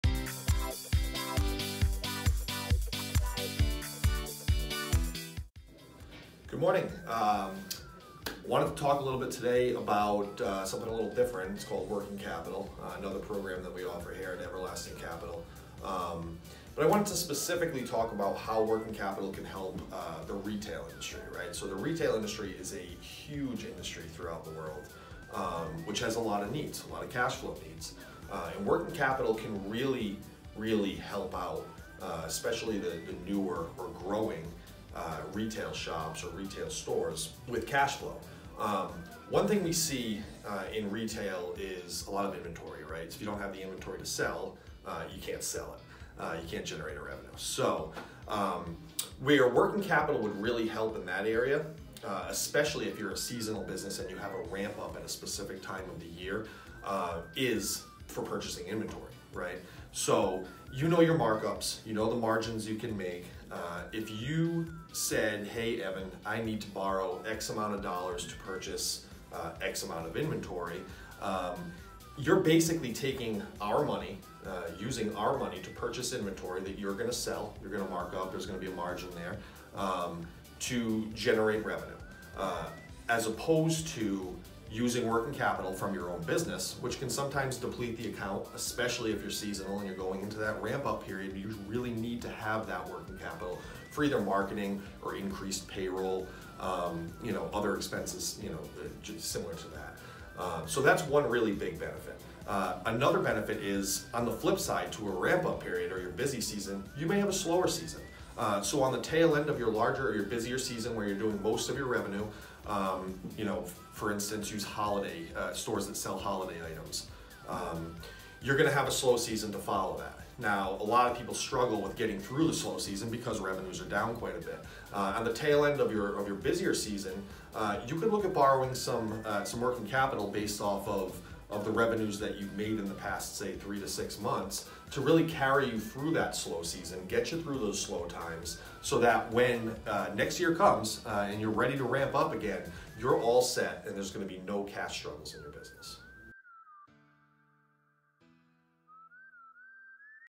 Working Capital For The RETAIL Industry!
When it comes to owning a retail business, we understand some of the struggles that come along with that business. We know ends don’t always meet, revenue isn’t always steady. Everlasting Capital is here to make those ends meet and exceed! We want nothing more than for your success and growth!

3 Things to take away:
1. Use Working Capital to cover Marketing, Increased payroll, and many other expenses!

2. Why use your businesses hard earned money to cover expenses when you could use ours right now!

3. During a businesses slow season use Working Capital to recover and come back stronger!